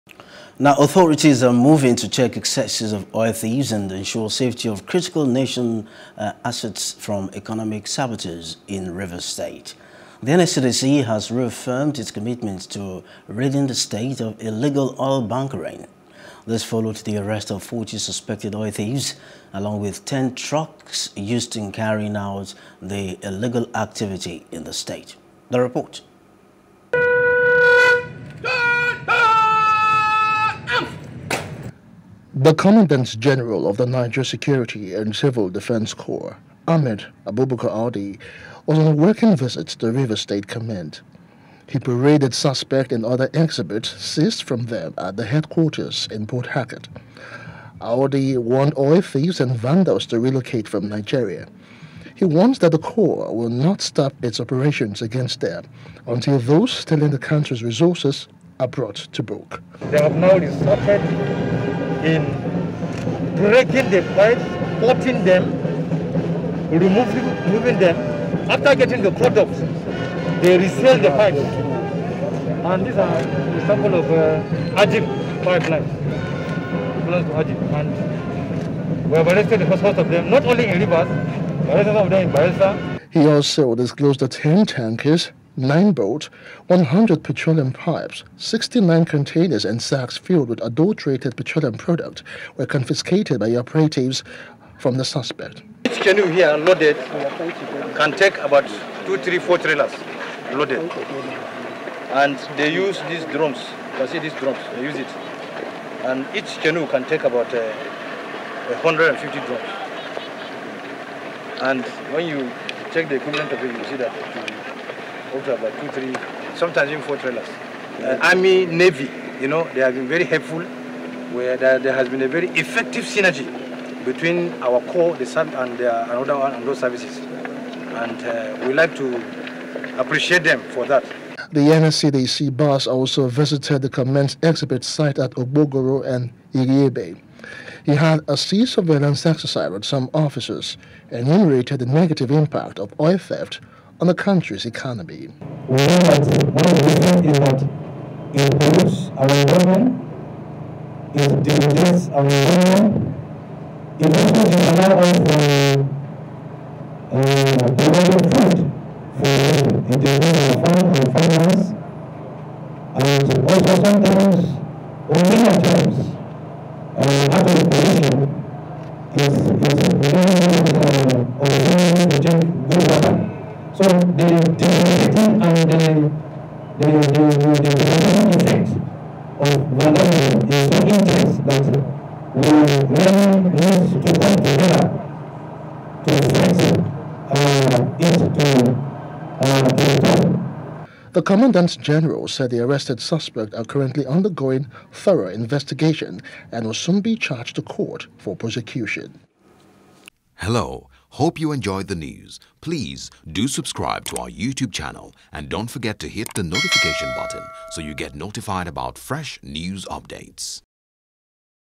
Rivers Oil Theft: N.S.C.D.C. Arrests 40 Suspects, Seizes 10 Truckloads | NEWS
Authorities are moving to check excesses of oil thieves and ensure safety of critical national assets from economic saboteurs in Rivers State.